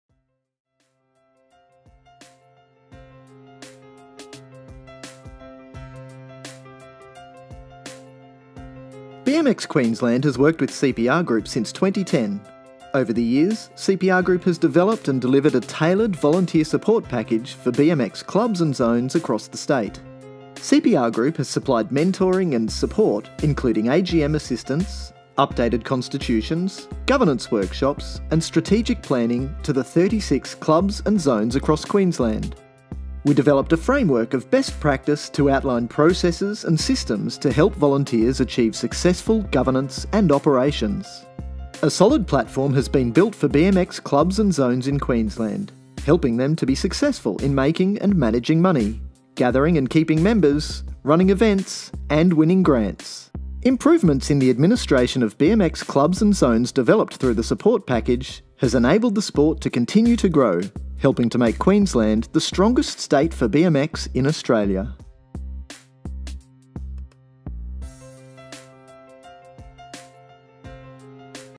190603 BMXQ Volunteer Support
CPR Group Project Profile

BMX Queensland Volunteer Support Package
Supporting volunteers since 2010

BMX Queensland has worked with CPR Group since 2010. Over the years, CPR Group has developed and delivered a tailored volunteer support package for BMX clubs and zones across the state.

CPR Group has supplied mentoring and support including AGM assistance, updated constitutions, governance workshops and strategic planning to the 36 clubs and zones across Queensland. We developed a framework of best practice to outline processes and systems to help volunteers achieve successful governance and operations.

A solid platform has been built for BMX clubs and zones in Queensland, helping them to be successful in making and managing money, gathering and keeping members, running events and winning grants.

Improvements in the administration of BMX clubs and zones developed through the support package has enabled the sport to continue to grow, helping to make Queensland the strongest state for BMX in Australia.